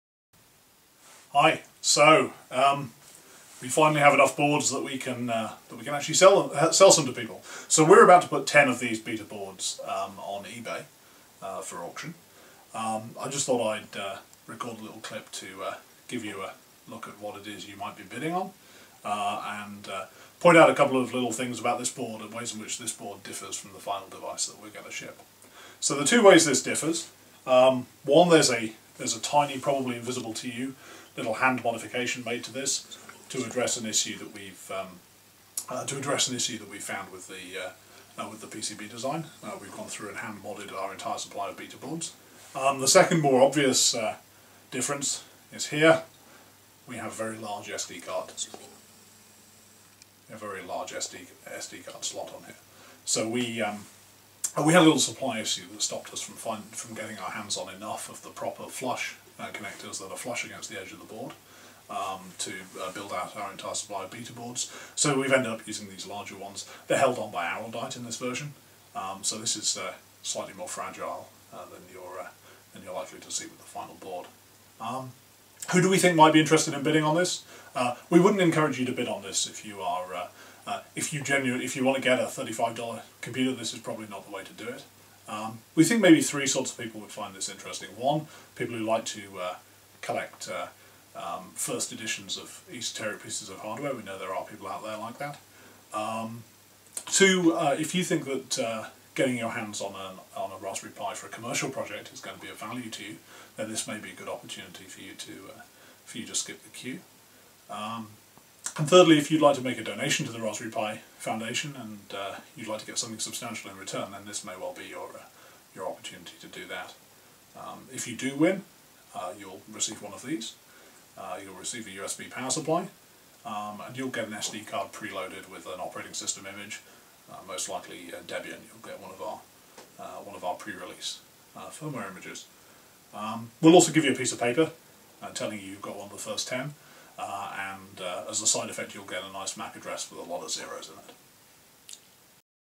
Raspberry Pi Model B beta boards ready for auction
We're auctioning ten Raspberry Pi beta boards. Eben talks us through the features of the new board.